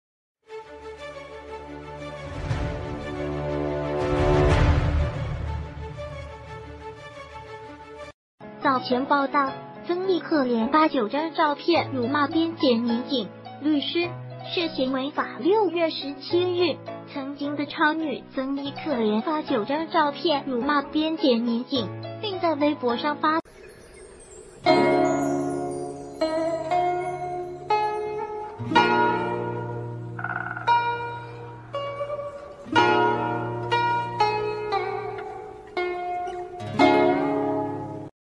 曾軼可連發9張照片辱罵警察 北京邊檢回應
早前報道：曾軼可連發...